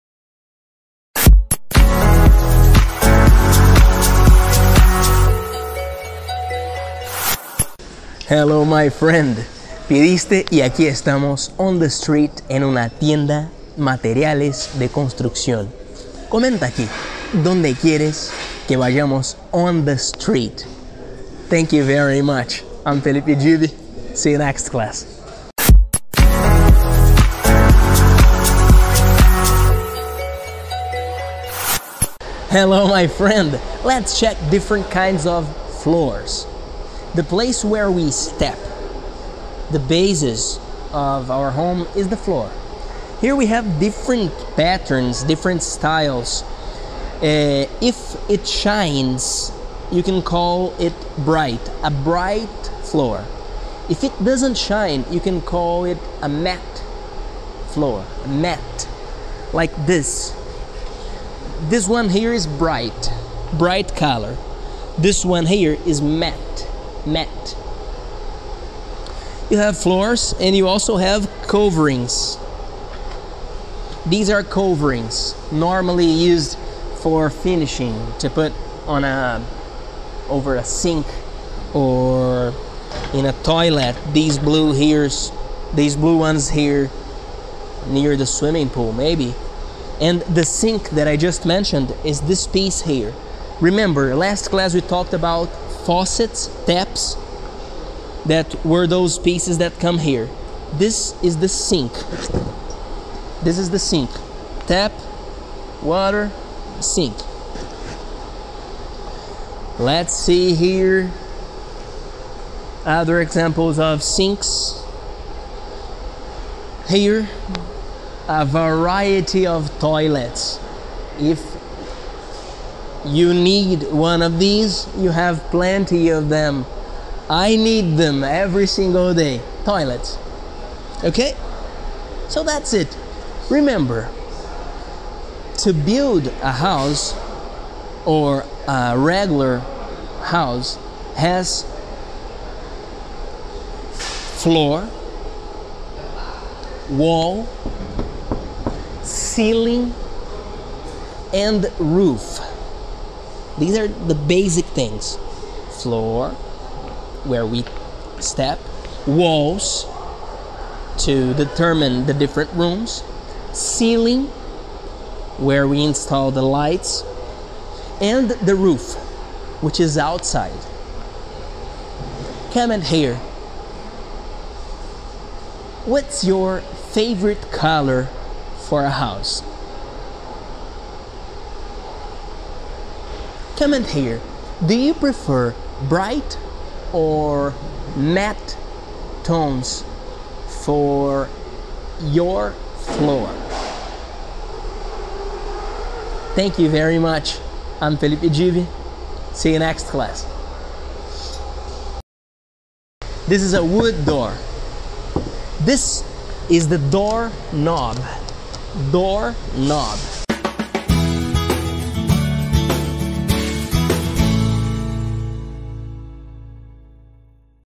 69- At a warehouse 5
Clase del curso de inglés y liderazgo tú aprendes ahora on the street (en la calle). Let's check different kinds of floors (vamos a revisar diferentes tipos de pisos). The place where we step (el lugar que pisamos). The basis of our home is the floor (la base de nuestra casa es el piso). Here we have different patterns, different styles (aquí tenemos diferentes modelos, diferentes estilos). If it shines you can call it bright (si brilla puedes llamarlo brilloso). A bright floor (un piso brilloso). If it doesn't shine you call it matte (si no brilla, lo llamas mate). Like this (como este). This one here is bright (este de aquí es brillante). Bright color (color brillante). Matte (mate). You have floors and you also have coverings (tienes pisos y también tienes revestimientos). These are coverings normally used for finishing (estos son revestimientos normalmente usados para acabado). To put over a sink or in a toilet (para colocoar encima de un lavabo o en un baño). These blue ones here near the swimming pool, maybe (estos azules de aquí cerca de la piscina, talvez). The sink that I just mentioned is this piece here (el lavabo que acabe de mencionar es esta pieza de aquí). Last class we talked about faucets, taps that were those pieces that come here (la ùltima clase hablamos sobre llaves/grifos que eran esta piezas que ven aquí). This is the sink (este es el lavabo). Tap (grifo/llave). Water (agua). Sink (lavabo). Let's see here other examples of sinks (vamos a ver aquí otros ejemplos de lavabo). Here, a variety of toilets (aquí una variedad de sanitários). If you need one of these, you have plenty of them (si necesitas uno de estos, tienes muchos de ellos). I need them every single day (necesito de ellos todos los días). Toilets (sanitários). Remember (recurda). To build house, or a regular house has floor, wall, ceiling and roof (para construir una casa o una casa normal tien piso, pared, techo y tejado). These are the basic things (estas son las cosas básicas). Floor where we step (piso donde pisamos). Walls to determine the different rooms (paredes para determinar las diferentes salas/cuartos). Ceiling, where we install the lights and the roof, which is outside (techo, donde instalamos las luces e el tejado, que está del lado de afuera). Comment here (comenta aquí). What's your favorite color for a house? (¿cuál es tu color favorito para una casa?). Comment here (comenta aquí). Do you prefer bright or matte tones for your floor? (¿prefieres tonos brillantes o mates para tu piso?). See you next class!